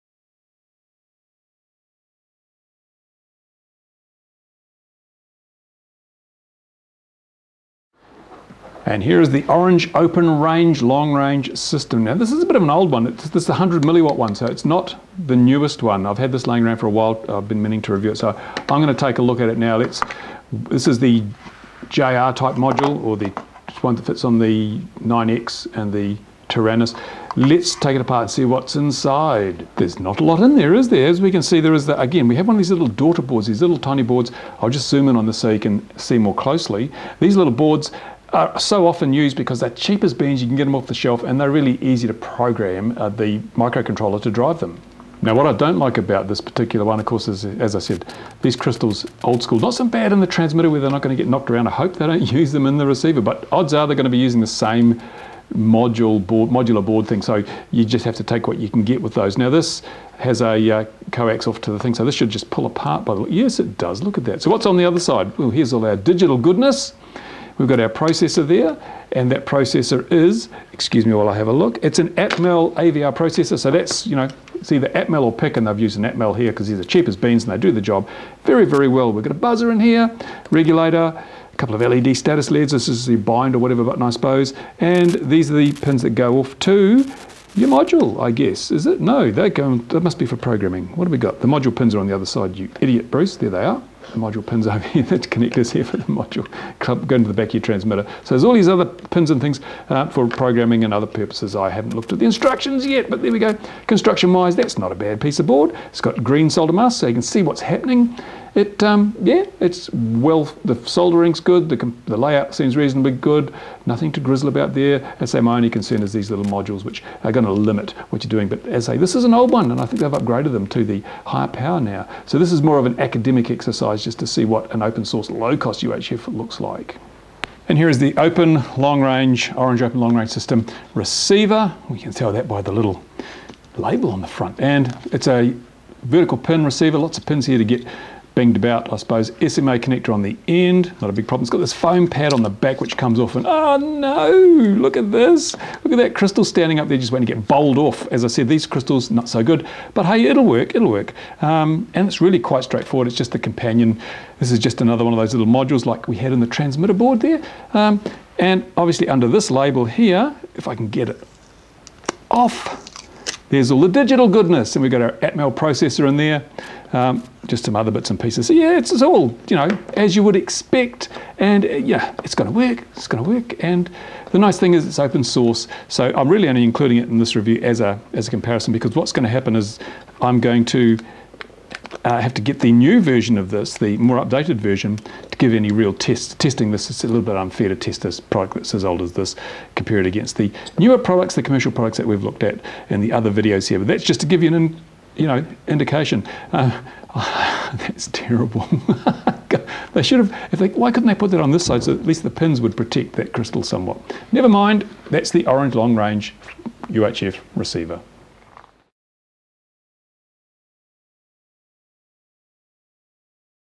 UHF RC Shootout: Open LRS Long Range System (part 1)
This is the Orange Open LRS long range UHF RC system.

This video (part 1) looks as the general design, layout and quality of construction and it should be noted that this is an older version of the product, included mainly for comparison purposes.

The Orange LRS transmitter is a simple but well made product.  The PCB is well laid-out and the soldering is of acceptable quality.  The RF module used is only 100mW and uses old-school crystals.
From a hardware perspective, this system gets a rating of good.

The receiver is compromised by the clunky module (with its old-school crystal).   The receiver scores a poor rating from a hardware perspective.

Pro's:
 - Nice plastic molding for the JR-type transmitter module
 - Simple design and layout

Con's:
 - very low-spec RF module in transmitter and receiver
 - receiver design prone to damage due to module positioning

Check part 2 for the bench-tests which will determine how the system actually performs.